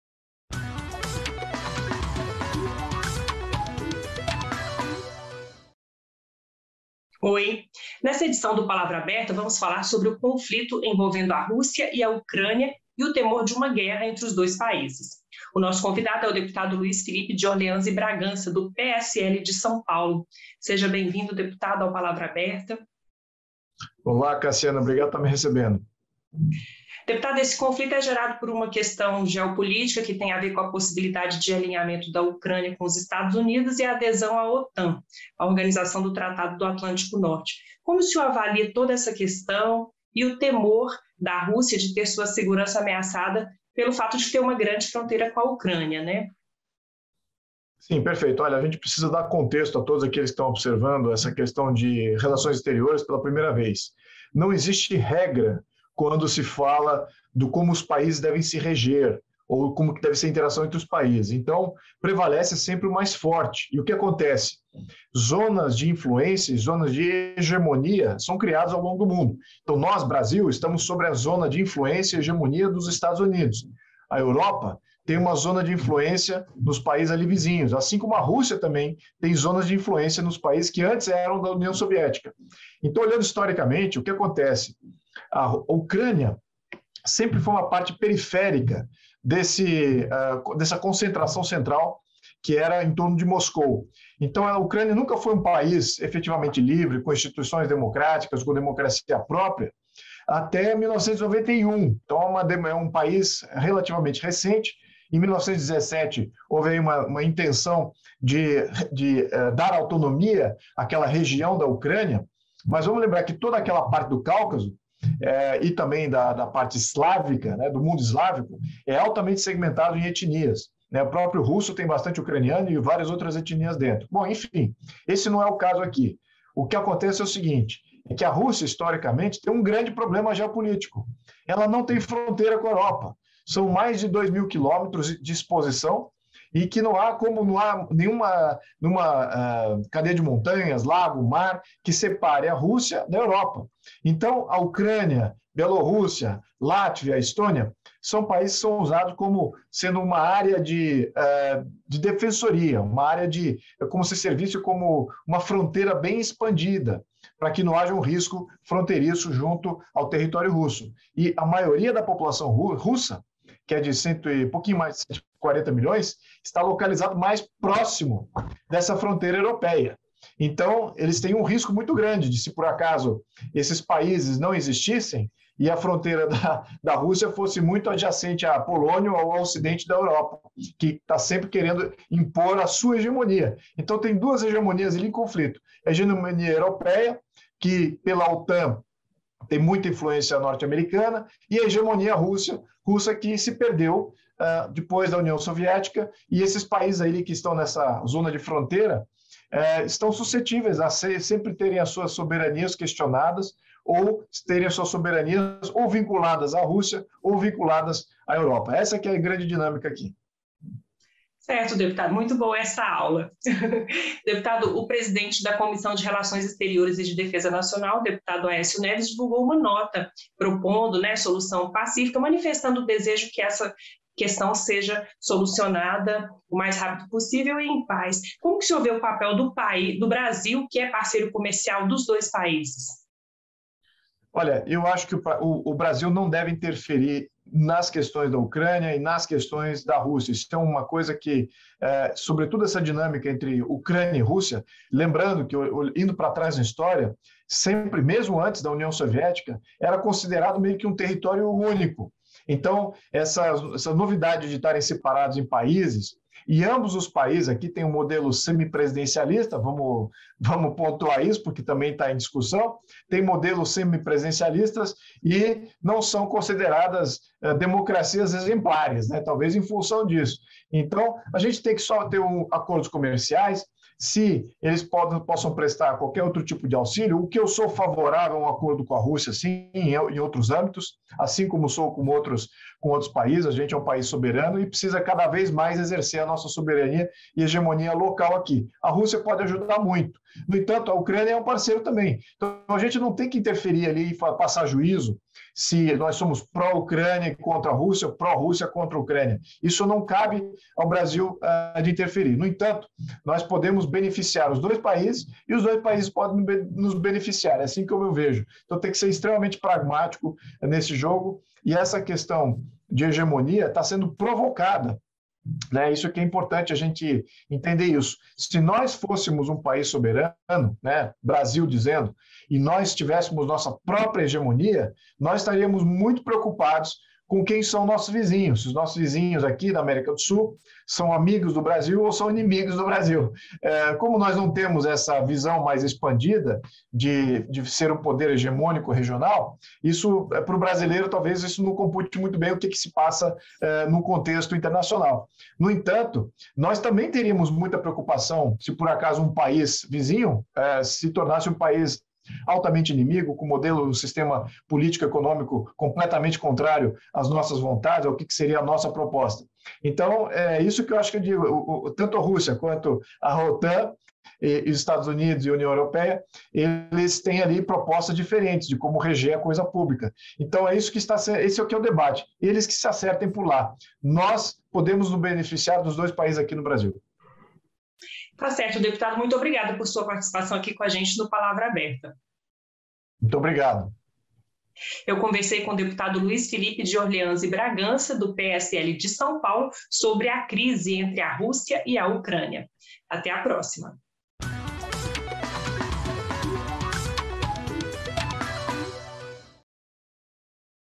Luiz Philippe de Orleans e Bragança comenta a crise entre Rússia e Ucrânia - 18/02/22
Nessa edição do Palavra Aberta vamos falar sobre o conflito envolvendo a Rússia e a Ucrânia, e o temor de uma guerra entre os dois países. O nosso convidado é o deputado Luiz Philippe de Orleans e Bragança (PSL-SP).

Apresentação: Cassiana Tormin - Direção: Guga Caldas